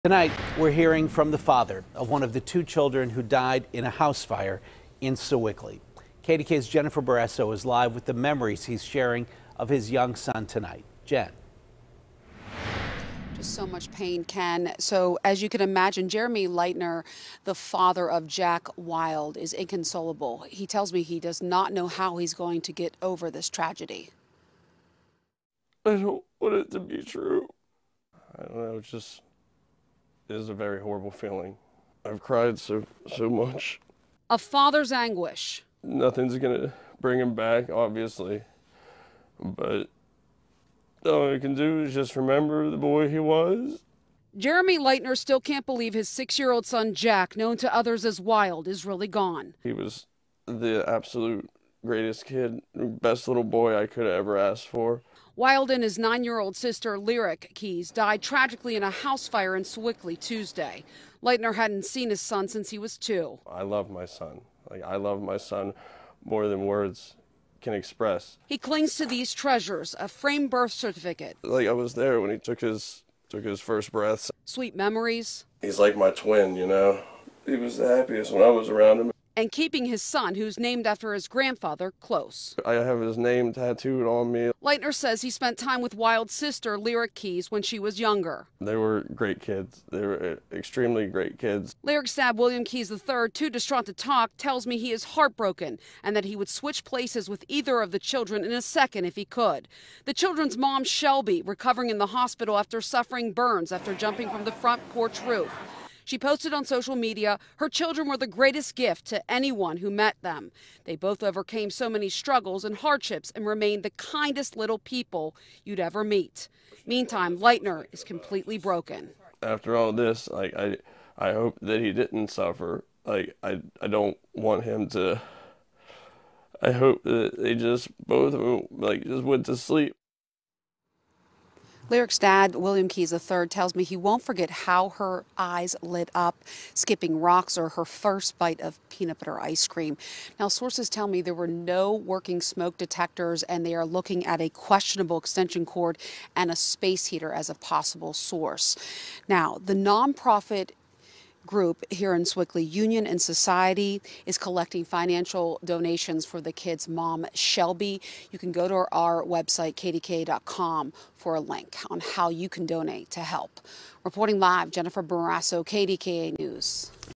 Father remembers 6-year-old son killed in Sewickley fire
The father of one of the children who died in a house fire in Sewickley is talking about his son; KDKA's Jennifer Borrasso reports.